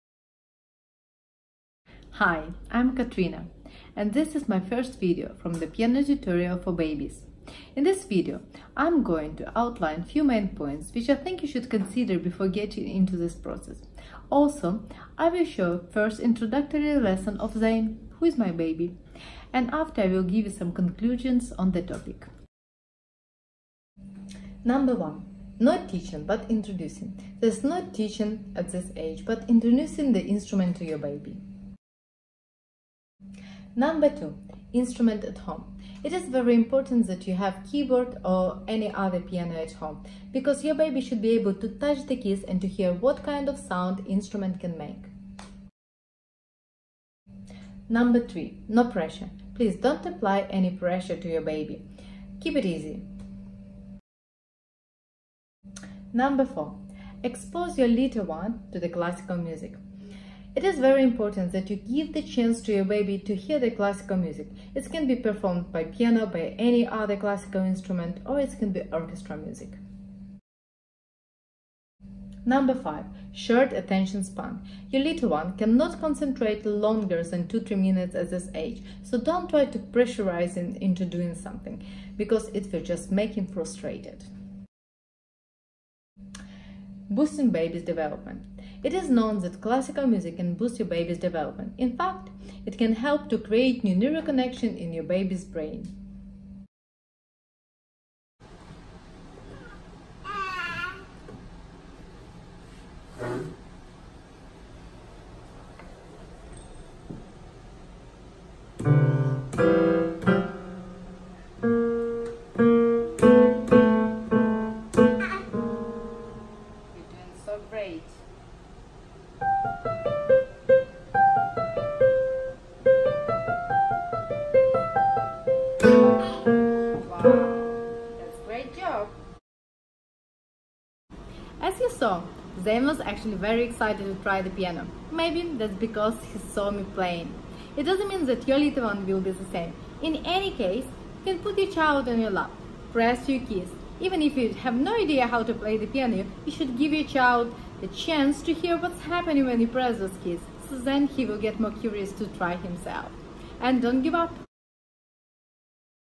Piano tutorial for babies. Part 1. Introduction.
In this video I’m describing few main points to consider before introducing the piano to your baby. Showing Zayn’s first trial on the piano. Giving supportive tips.